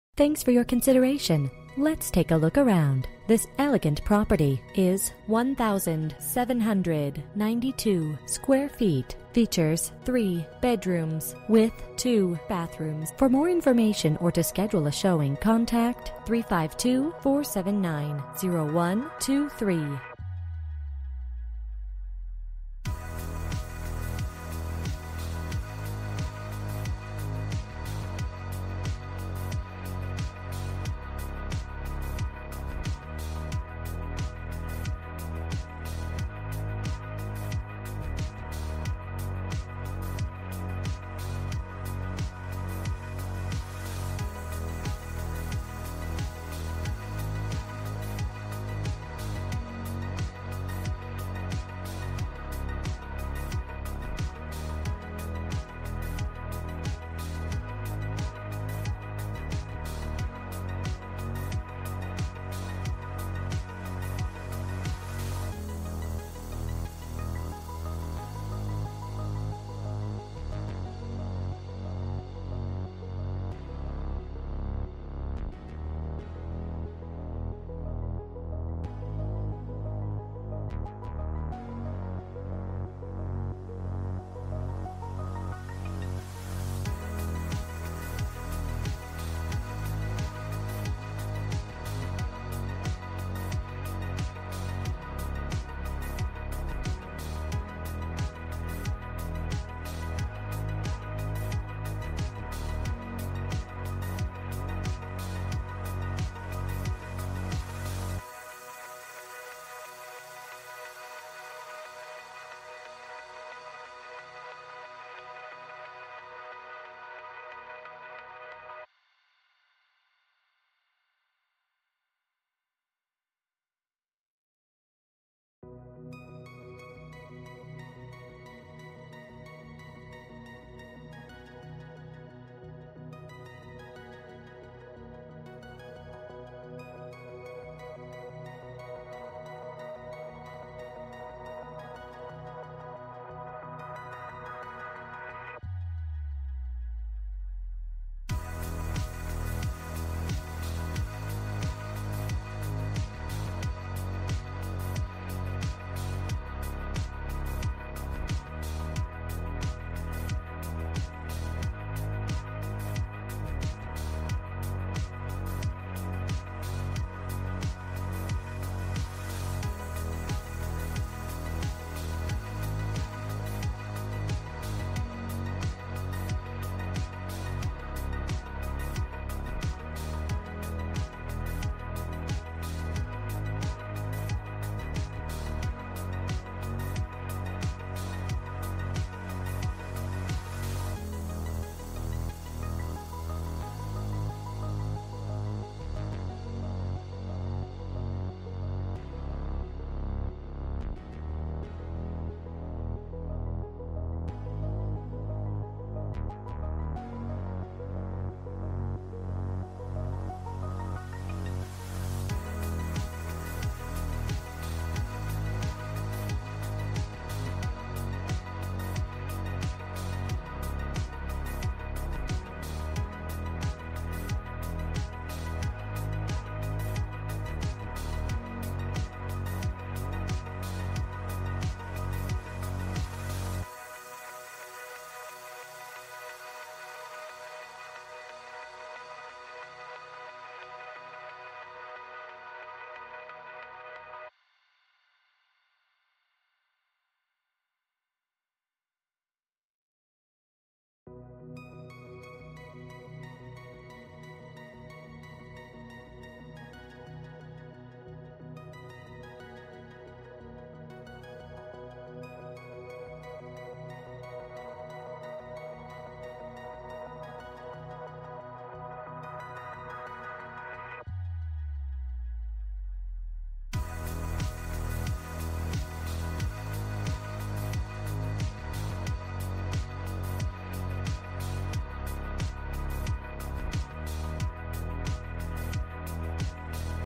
10979 SW 81ST AVENUE for sale in OCALA, FL 34481 - Residential
This popular well maintained Williamsburg model with many upgrades will ease your eyes when you walk into the front door to the large living room with cathedral ceiling. Three spacious bedrooms with remodeled master bath and walk in shower. The kitchen will speak for itself with newer cabinets and solid counter tops, all stainless steel appliances all overlooking the spacious dining room. Step into Mr Cleans two car garage with washer and dryer. The 200 amp electrical service and roof were just replaced in 2022. Furniture and golf cart can be negotiated with the owner.

Bedrooms: 3
Bathrooms: 2
Square Feet: 1792
Price: $279,900

MLS ID: OM660889